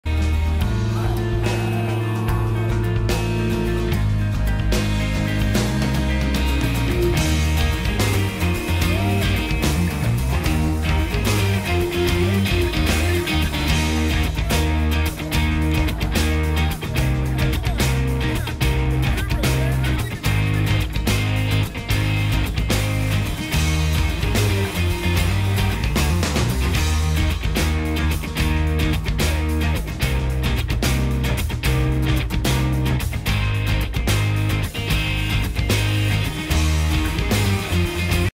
Mountaineer Challenge Academy Class 2-2018 Vehicle Relocation
New Candidates work together to pull a truck